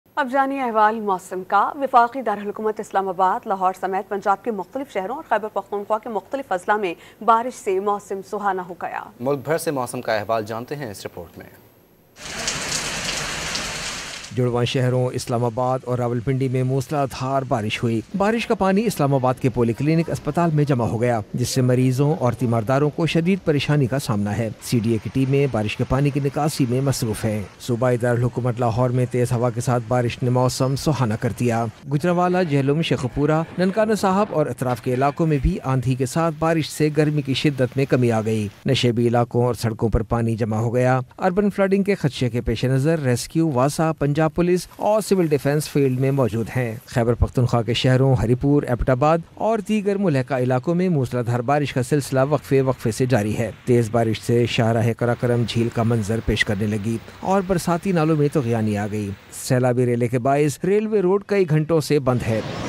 Rain In Various Districts Of Islamabad, Punjab And Khyber Pakhtunkhwa | Dawn News
اسلام آباد، پنجاب اور خیبرپختونخوا کے مختلف اضلاع میں بارش